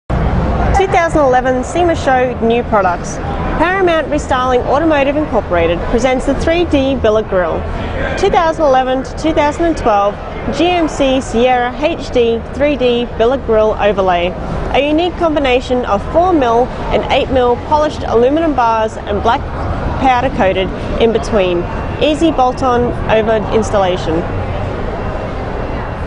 3D Billet Grille from Paramount Restyling Automotive Inc. ID13136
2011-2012 GMC Sierra HD 3D Billet Grille overlay. A unique combination of 4mm and 8mm polished aluminum bars and black powder coated in-between. Easy bolt over installation.